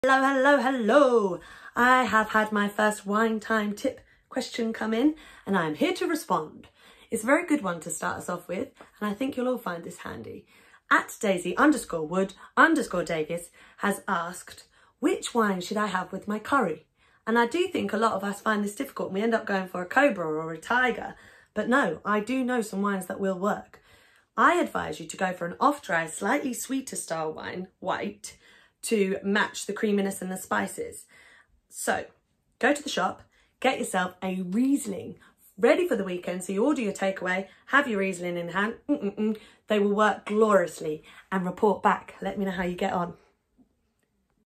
Wine Time Tips WINE WITH CURRY
❓Which wine should I match with curry? From @daisy_wood_davis
Riesling❕
Off-dry sweeter style white wines are the best match for spicier food. Try a German or Austrian Riesling and let me know how you like it! X